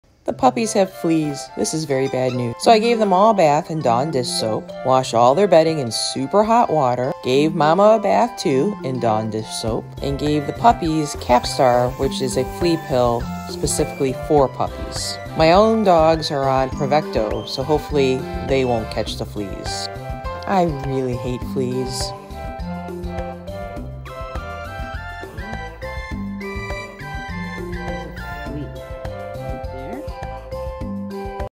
Killing fleas
When you have puppies too young to get a flea treatment but they have fleas, give them a bath in Dawn dish soap.  You can also put dawn in a lawn sprayer and spray the yard to kill fleas in the grasse,